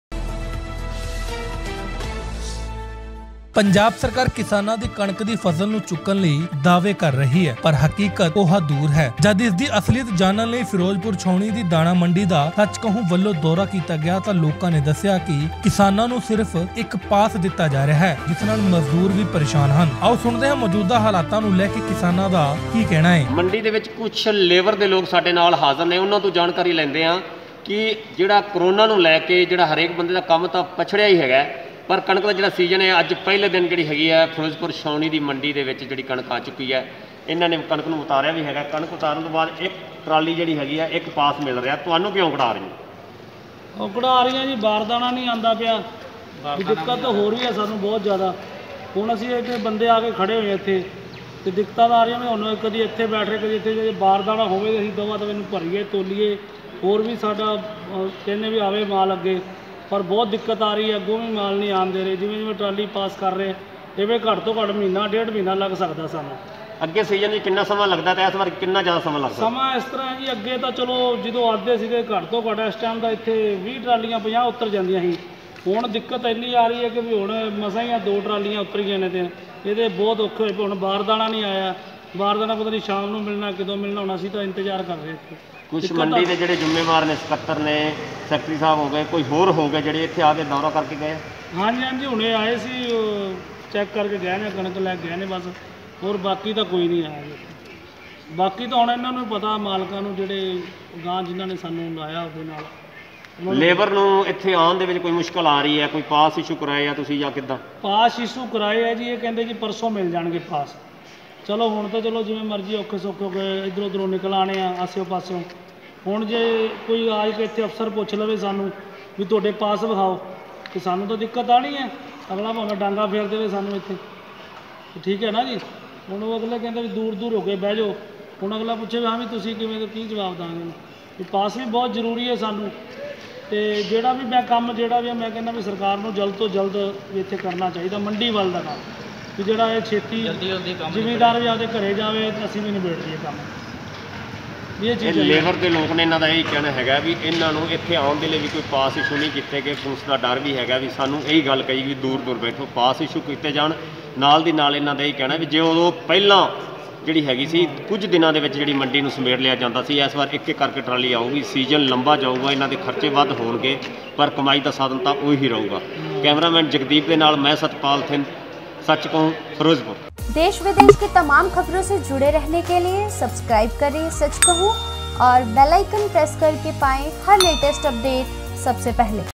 Ferozepur || ਕੀ ਪੰਜਾਬ ਸਰਕਾਰ ਨੂੰ ਹੈ ਕਿਸਾਨਾਂ ਦੀ ਚਿੰਤਾ ?
ਪਜਾਬ ਸਰਕਾਰ ਨੇ ਕਿਸਾਨਾਂ ਦੀ ਕਨਕ ਦੀ ਫਸਲ ਨੂੰ  ਚੁੱਕਣ ਲਈ ਦਾਅਵੇ ਕਰ ਰਹੀ ਹੈ ਪਰ ਹਕੀਕਤ ਕੋਹਾ ਦੂਰ ਹੈ ।
ਜਦ ਇਸ ਦੀ ਅਸਲੀਅਤ ਜਾਨਣ ਲਈ ਫਿਰੋਜ਼ਪੁਰ ਛਾਉਣੀ ਦੀ ਦਾਣਾ ਮੰਡੀ ਦਾ ਸਚ ਕਹੂ  ਵੱਲੋਂ ਦੌਰਾ ਕੀਤਾ ਗਿਆ ਤਾਂ ਲੋਕਾਂ ਨੇ  ਦਸਿਆ ਕੇ ਕਿਸਾਨਾਂ ਨੁੰ ਸਿਰਫ ਏਕ ਪਾਸ ਦਿੱਤਾ ਜਾ ਰਿਹਾ ਹੈ ਜਿਸ ਨਾਲ ਮਜਦੂਰ ਵੀ ਪਰੇਸ਼ਾਨ  ਹਨ ।